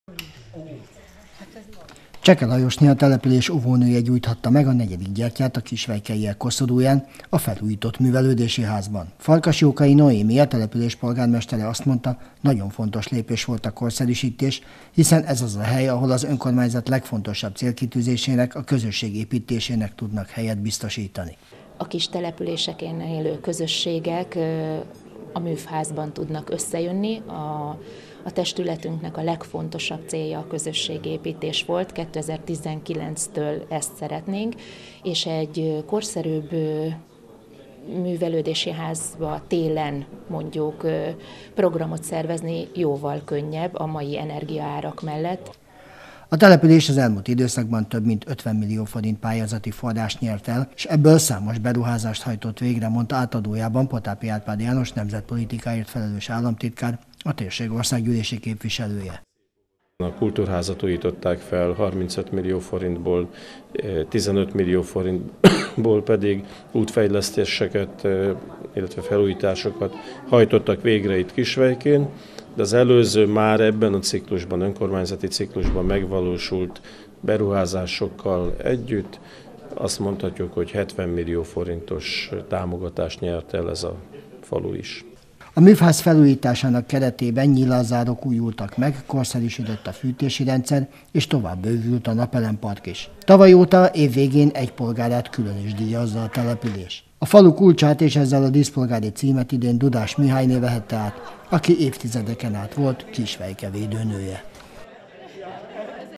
Művelődési házat avattak Kisvejkénn
Művelődési házat avattak Kisvejkén

Kettős ünnepet ültek advent negyedik hétvégéjén Kisvejkén. A gyertya meggyújtása egyben a megújult művelődési központ átadását is jelentette. Az elmúlt időszakban a település közel 50 millió forint pályázati forrást nyert, ami lehetőséget teremtett utcák burkolatának javítására, játszótér kialakítására. és a művelődési ház korszerűsítésére is.